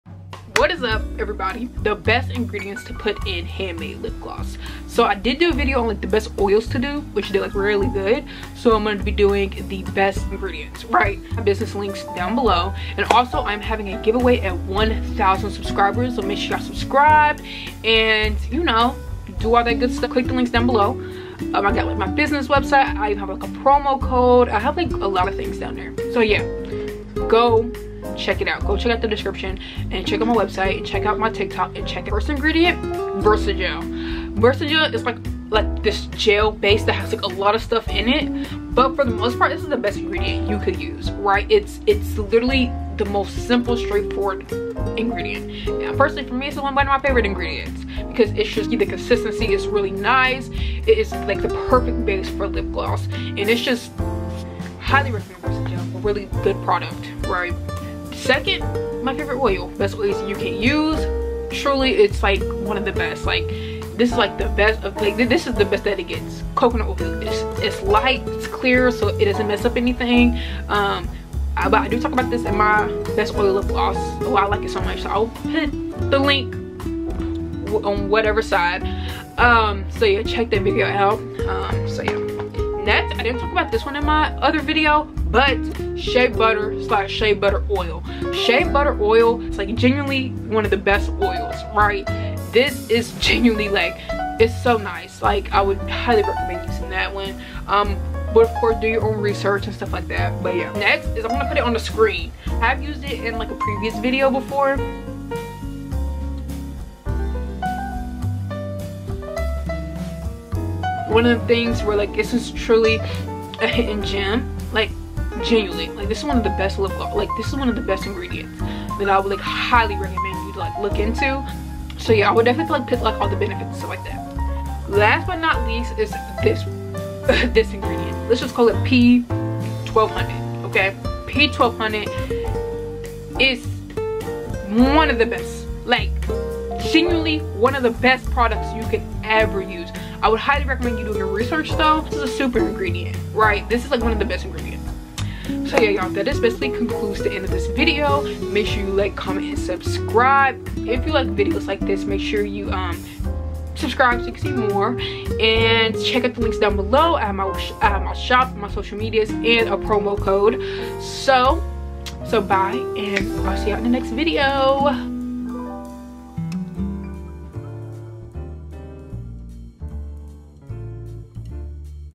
BEST LIPGLOSS INGREDIENTS | start a successful lipgloss business in *2024*
The best ingredients to put in handmade lipgloss. These are my favorite ingredients/ top ones I use for making lipgloss.

Hope you enjoy and learned something new!

Have any videos you would like to see?